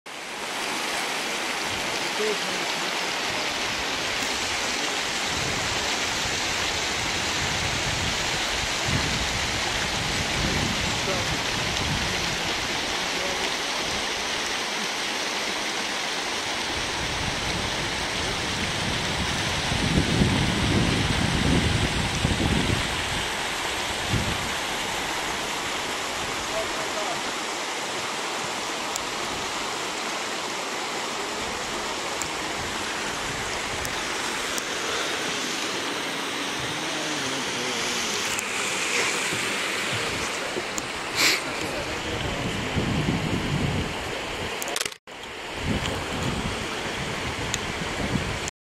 Huanglong National Park 7 - Beautiful Pools
Amazing cyan pools in Huanglong National Park, China 2008